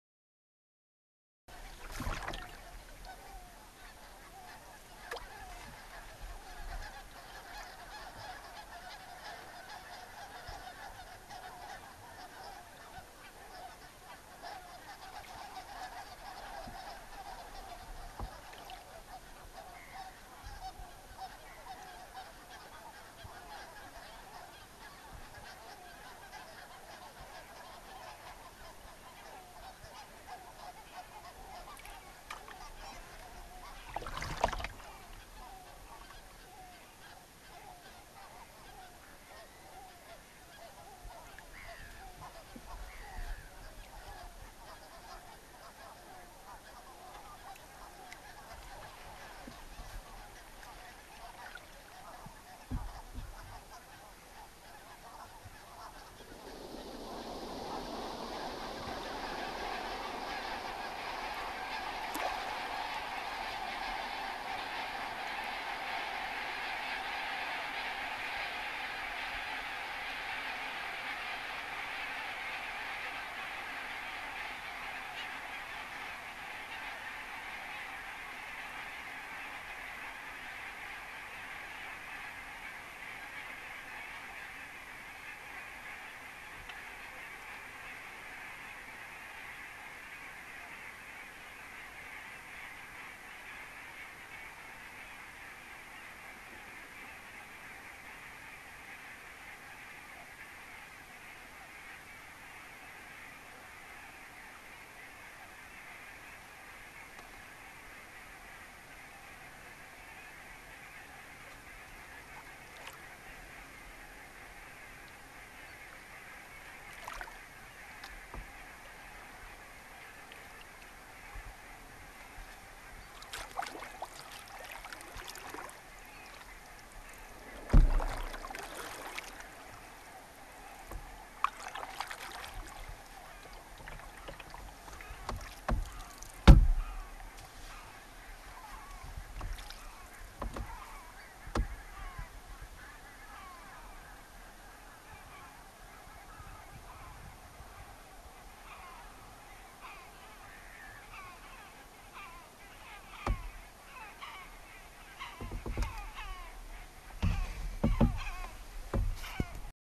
River Wampool geese.
Paddle on the River Wampool at Anthorn in Cumbria on 30th November 2014. Lots of Waterfowl about at dusk on the Solway. Paddling a Mad River Explorer 15rx.